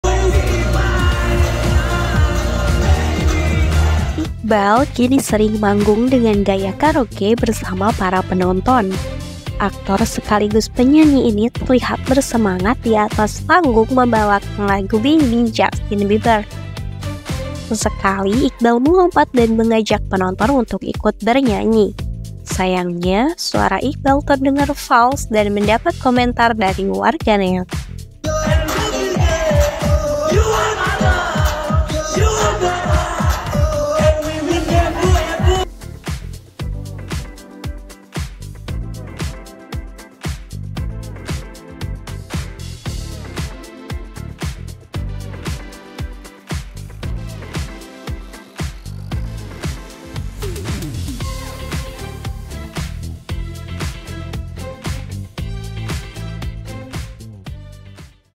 Nyanyikan Lagu Justin Bieber Suara Iqbaal Ramadhan Dirujak Netizen!
KAPANLAGI.COM - Aktor sekaligus penyanyi tampan, Iqbaal Ramadhan menjadi perbincangan warganet. Saat manggung, personil ex-CJR itu menyanyi dengan semangat, namun suaranya terdengar fals.